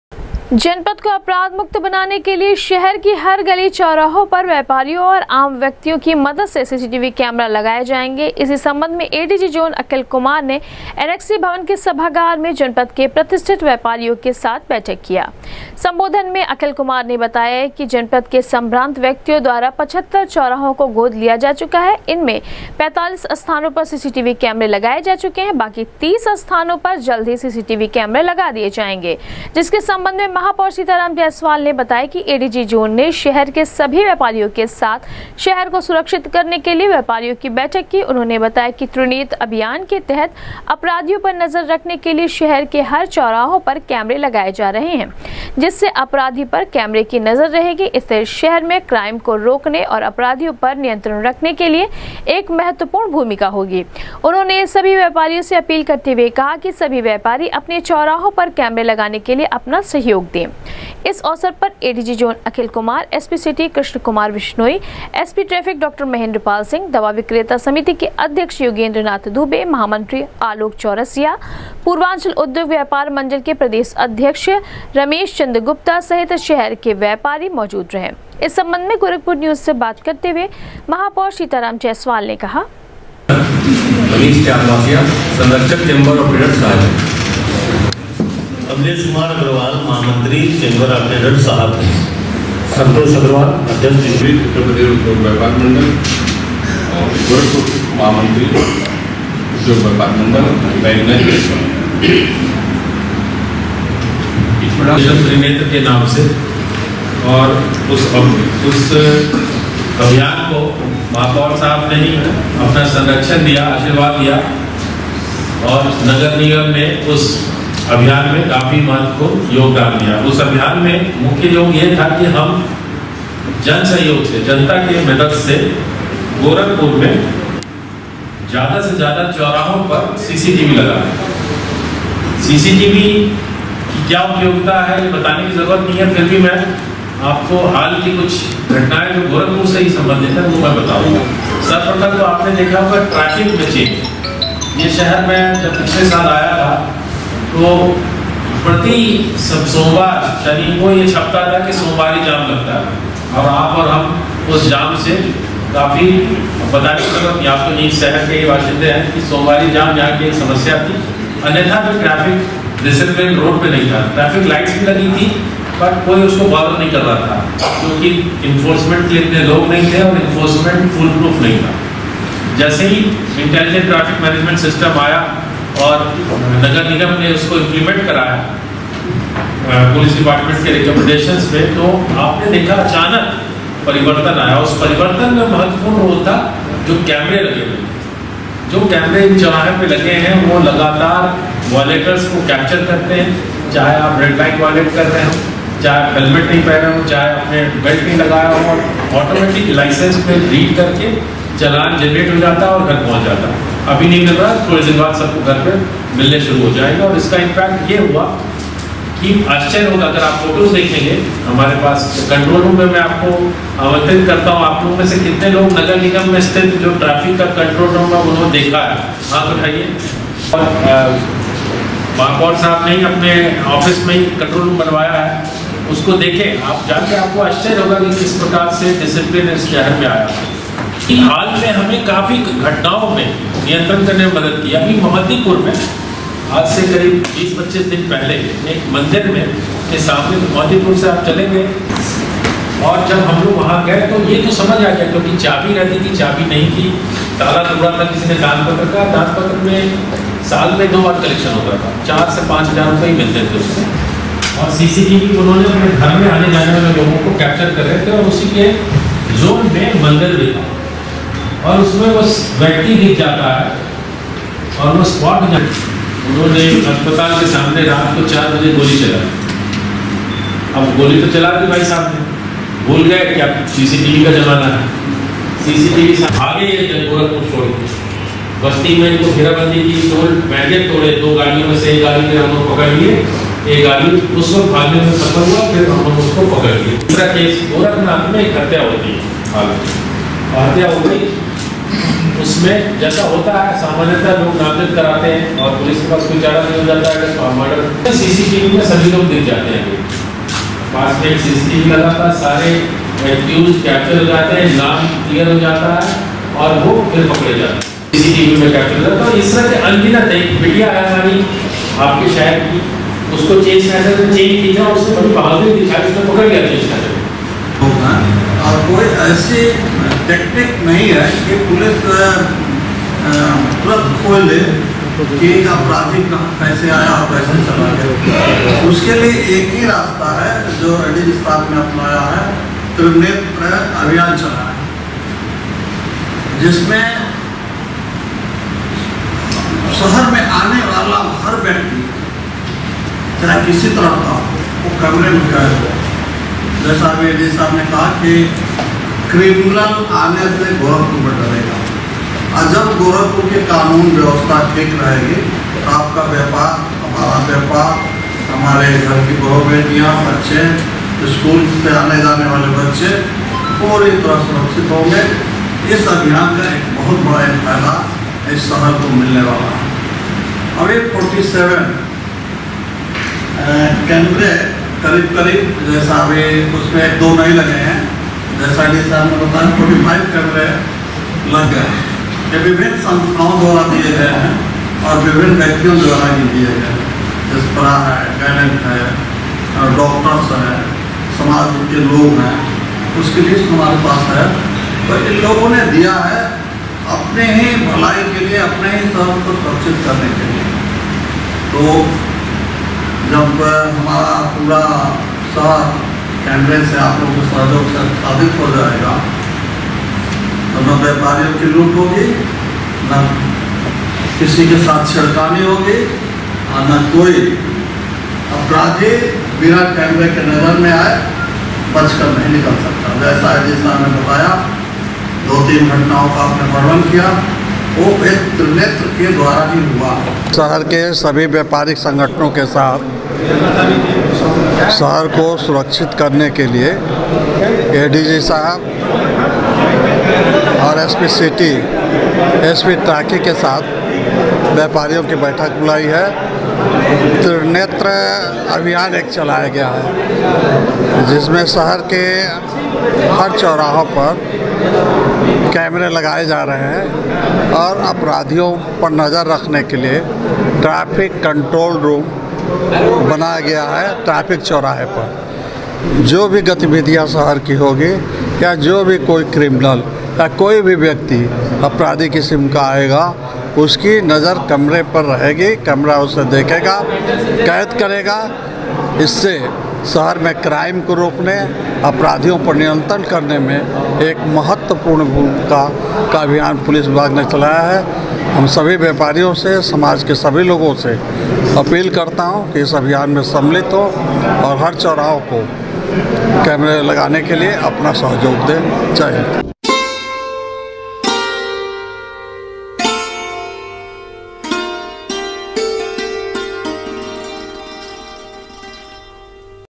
जनपद को अपराध मुक्त बनाने के लिए त्रिनेत्र अभियान के तहत शहर की हर गली चौराहो पर व्यापारियों और
जनपद को अपराध मुक्त बनाने के लिए त्रिनेत्र अभियान के तहत शहर की हर गली चौराहो पर व्यापारियों और आम व्यक्तियों की मदद से लगाए जाएंगे सीसीटीवी कैमरे, एडीजी जोन ने शहर के सभी व्यापारियों के संग की बैठक,कहा, क्राइम को रोकने और अपराधियों पर नियंत्रण रखने के लिए सीसीटीवी कैमरो की है महत्वपूर्ण भूमिका